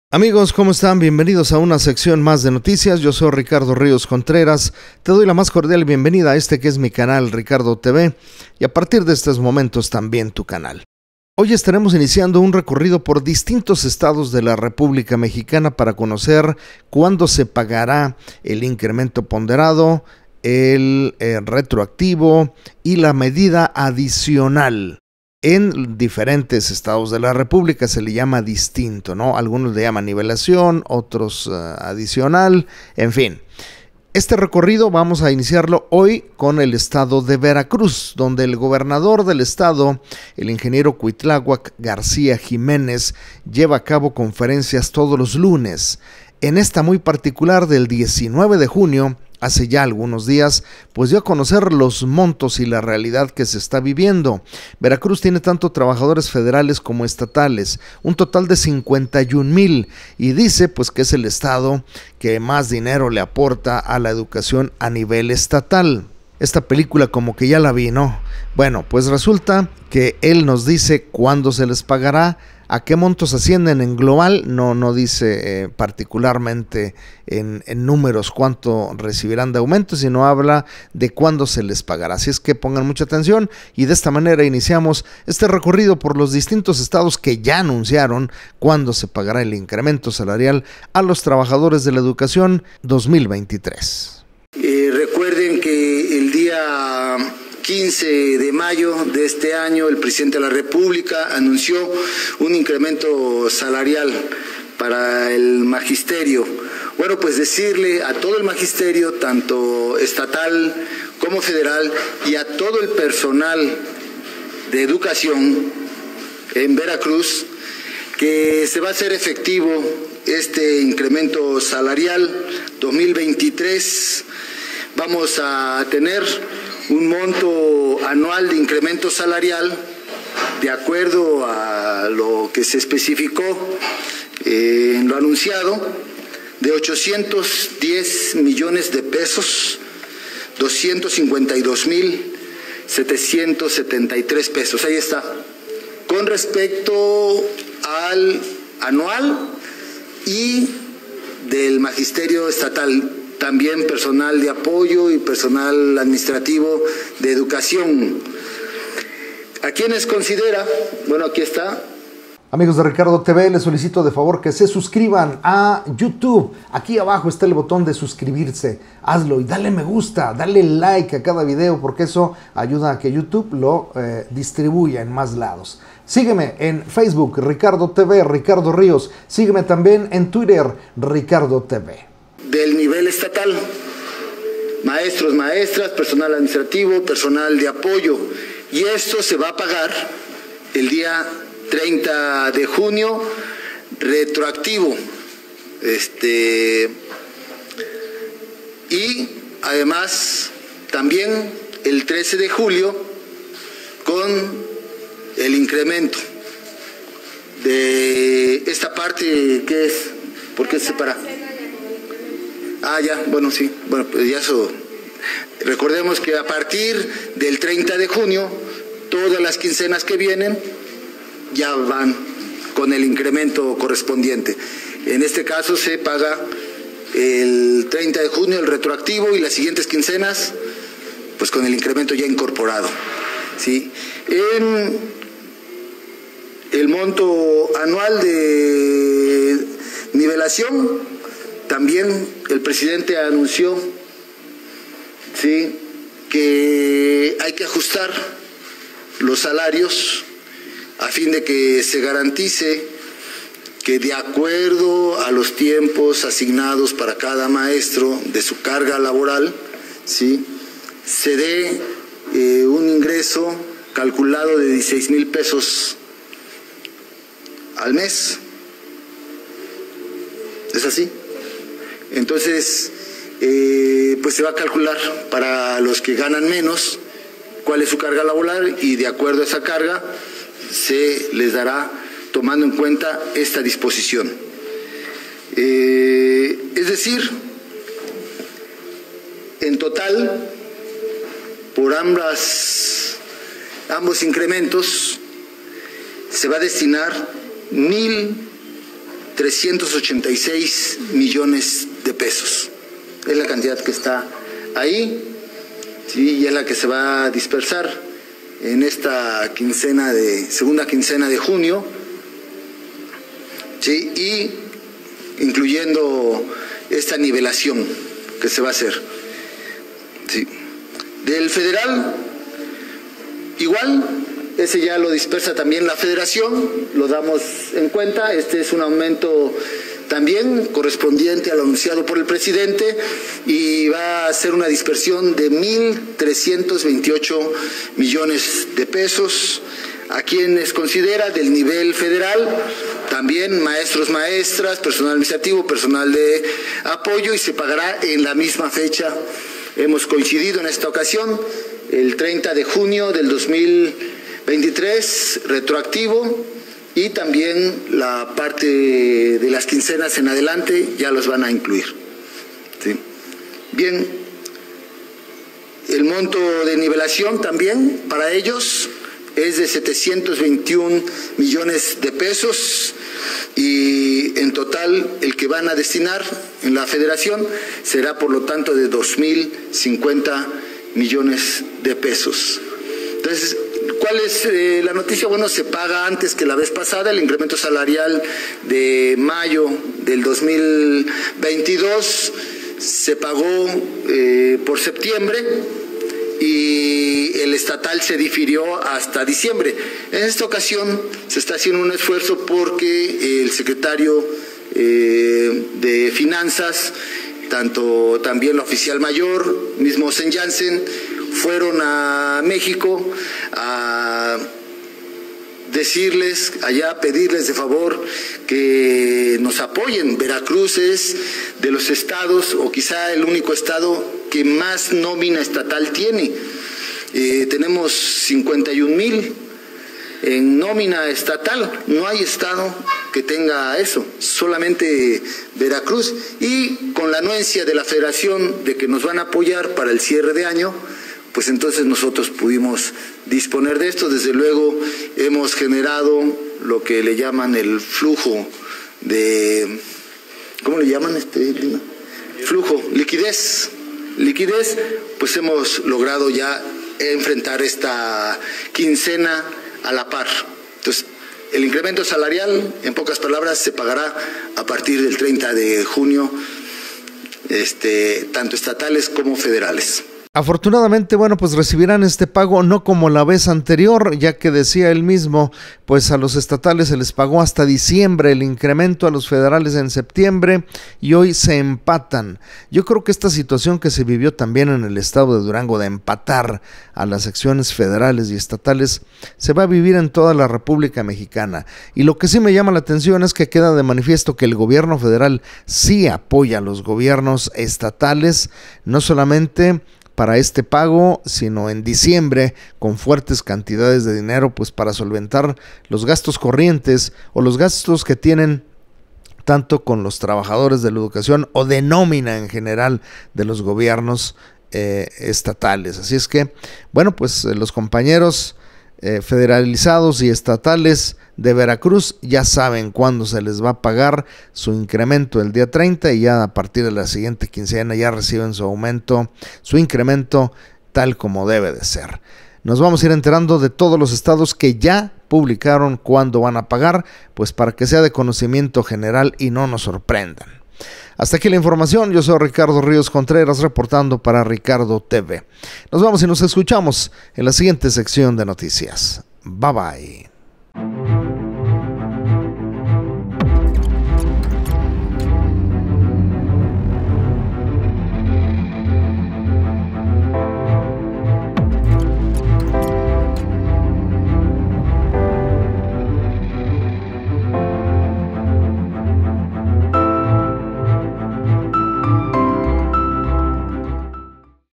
Cuándo pagan en Veracruz el Incremento a Trabajadores de la educación
Inicia un recorrido por varios Estados de la República Mexiana para conocer cuándo se paga el incremento salarial a los trabajadores de la educación, el retroactivo y las medidas adicionales o ajustes pertinentes para alcanzar los $16,000 anunciado por AMLO.